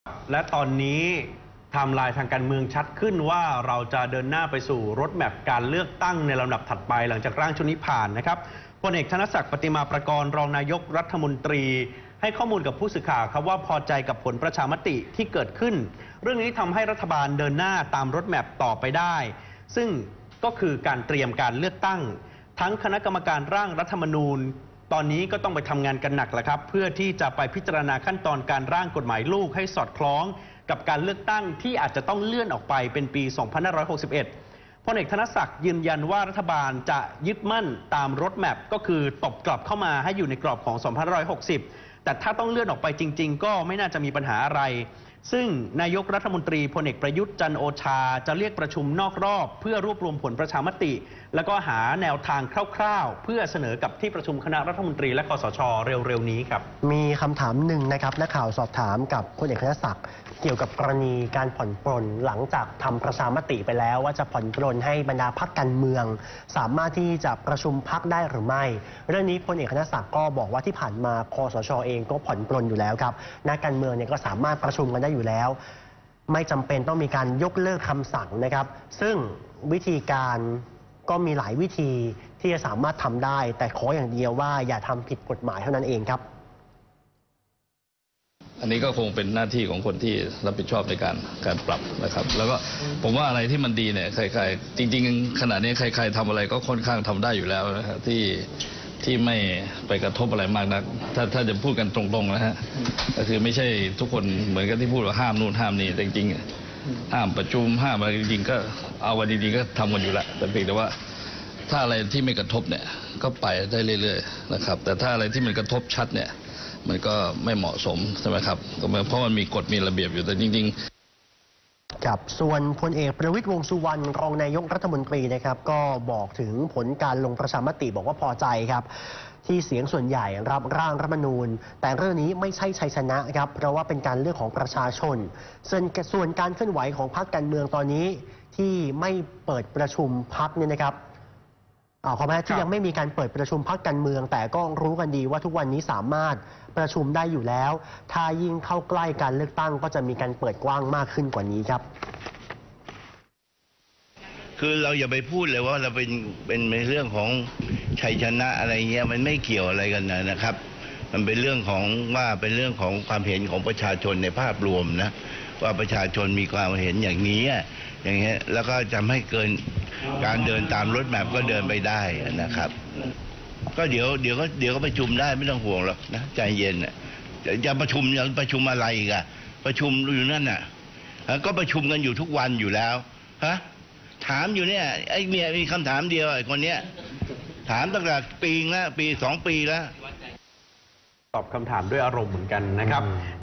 News รัฐบาล คสช.ย้ำเดินหน้าโรดแมพเลือกตั้งปี 60
พิกัดข่าวเที่ยง : เดินหน้าหลังลงประชามติ รัฐบาล คสช.ย้ำเดินหน้าโรดแมพเลือกตั้งปี 60 "NOW26" 8-8-59